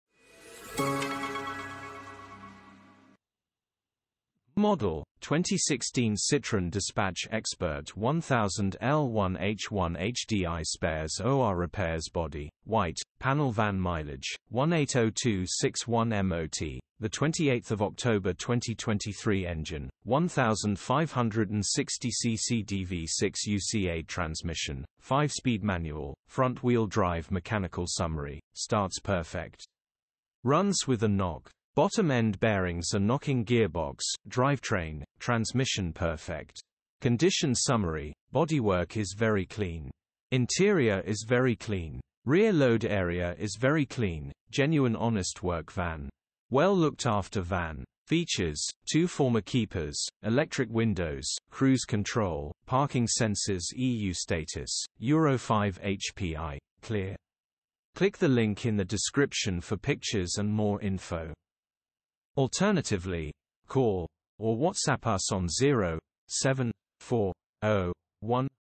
FH16AYL - 2016 CITROEN DISPATCH 1000 L1H1 HDI ()
Model: 2016 CITROEN DISPATCH EXPERT 1000 L1H1 HDI SPARES OR REPAIRS
Body: White - Panel Van
Mileage: 180261
MOT: 28th October 2023
Engine: 1560cc DV6UC-A
Transmission: 5 Speed Manual - FWD
Mechanical Summary: 🟠 Starts perfect. Runs with a knock. Bottom end bearings are knocking Gearbox, drivetrain, transmission perfect.
Features: 2 Former Keepers, Electric Windows, Cruise Control, Parking Sensors
HPI: Clear